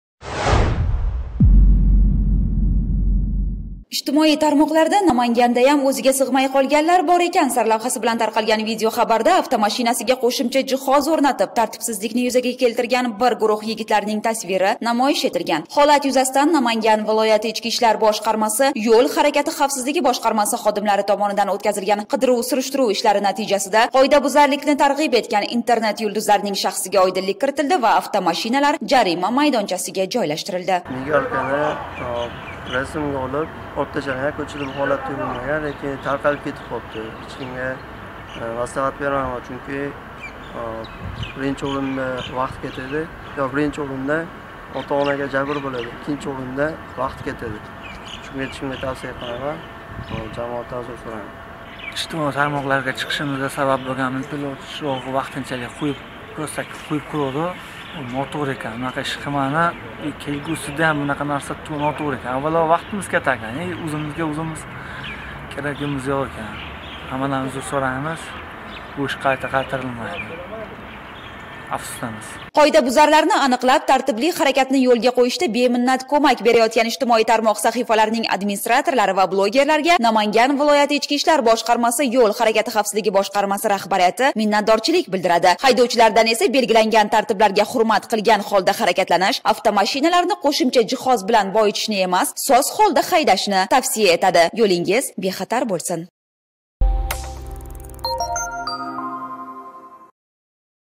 Ijtimoiy tarmoqda tarqalgan video o‘rganilib, qora rangli "Gentra"lar jarima maydoniga qo‘yildi
Namangan viloyatida ruxsat etilmagan mayoqchalar o‘rnatilgan, 50O007BA va 01312XHA davlat raqam belgili, qora rangli "Gentra"larning haydovchilariga nisbatan ma’muriy bayonnoma tuzilib, avtomobillar vaqtincha saqlash uchun jarima maydoniga qo‘yildi.
Barcha haydovchilardan qoidalarga rioya qilishlarini so‘raymiz.